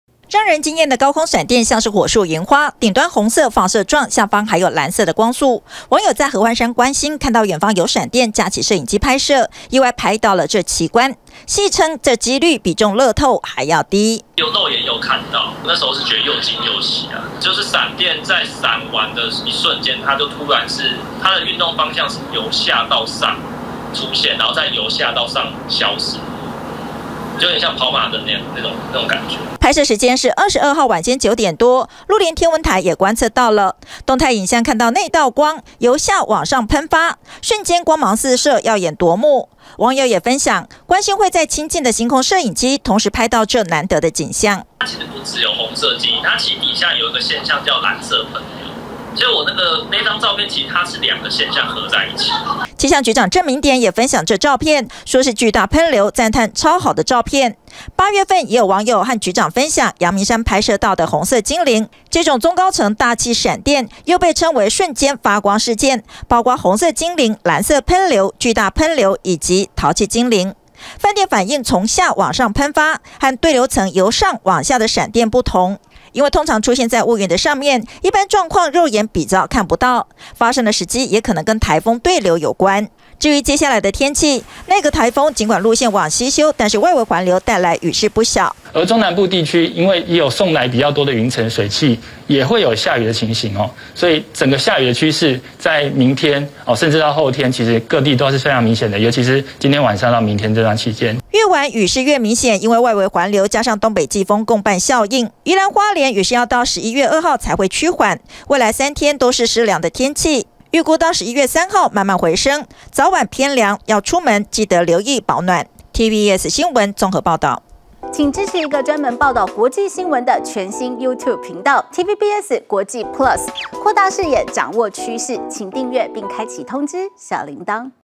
雲端焰火 合歡山觀星拍到紅色精靈+藍色噴流｜TVBS新聞
一名網友，在22號到合歡山觀星，拍下難得的天文景觀，猶如雲層上，往上噴發的閃電，上方看到"紅色精靈"，下方還有"藍色噴流"，鹿林天文台觀測，也拍到動態影像。